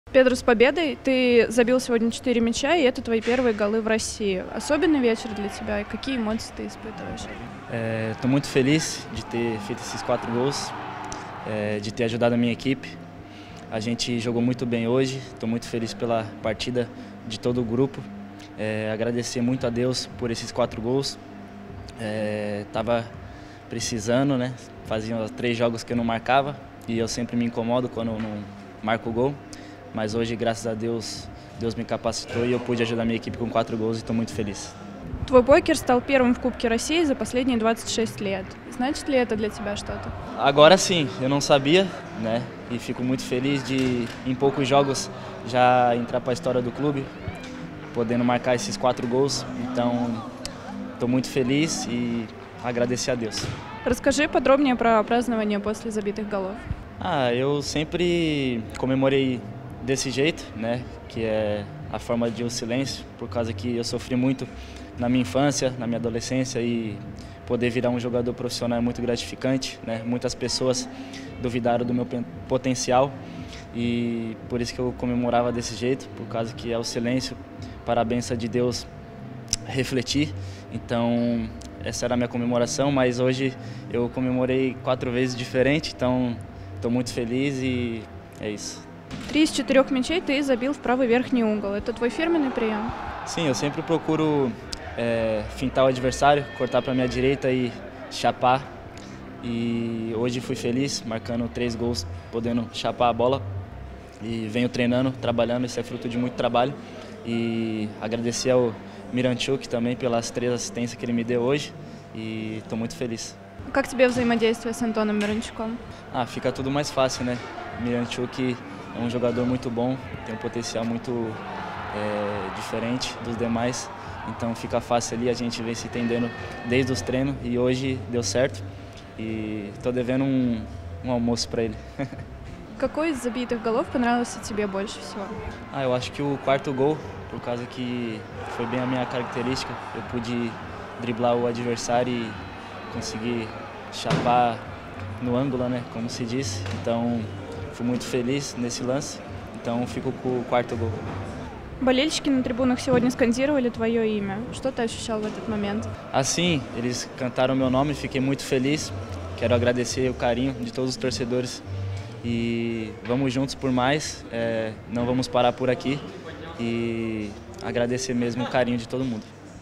Педро Габриэл: Теперь главное не останавливаться и двигаться дальше
Послематчевое интервью главного героя кубковой победы над «Химками» (5:0) — полузащитника «Локо» Педро Габриэла